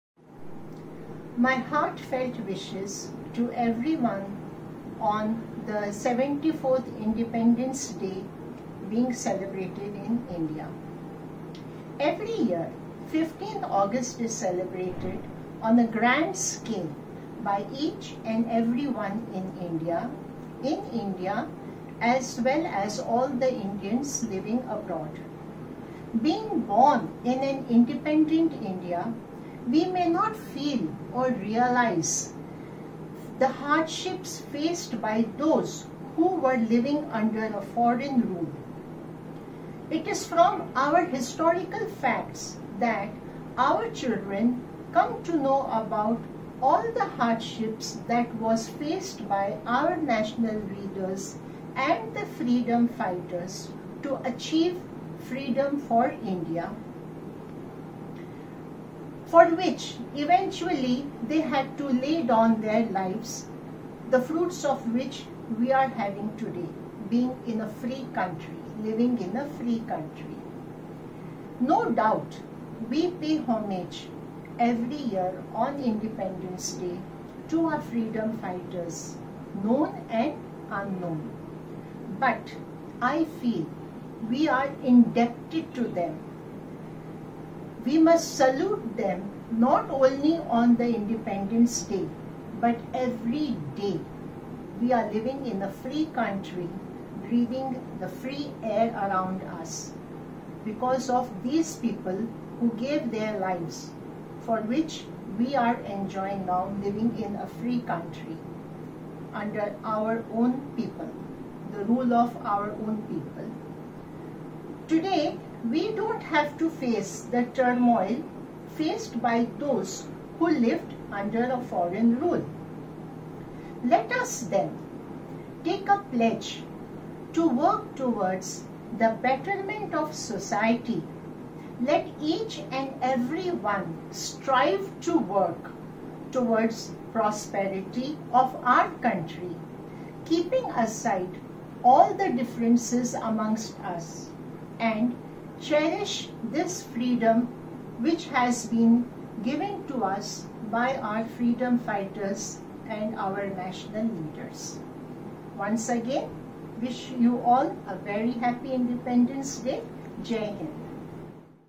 Independence Day Celebrations
“Freedom is never dear at any price. It is the breath of life. What would a man not pay for living?” – Mahatma Gandhi
This great thought by the father of the nation Mahatma Gandhi was the running theme of the Independence Day Celebrations held in Delhi School of Excellence, Banjara Hills on 15th August, 2020. The virtual celebrations brought to the fore the value of independence that we cherish and the struggle for freedom by the courageous efforts of the freedom fighters and their sacrifice. The celebrations were marked with myriad contributions by the students and the teachers in showcasing patriotism, creativity and imagination.
The students’ speeches, songs, dances, and artwork showed their love for the country and their desire to put forth the idea of India as ‘unity in diversity.’ The teachers let their songs, presentations, artwork and poetry speak volumes about patriotism. The students and teachers let their creativity and imagination soar in their contributions to make the program a grand success. Jai Bharat and Jai Hind. Happy Independence Day to all again !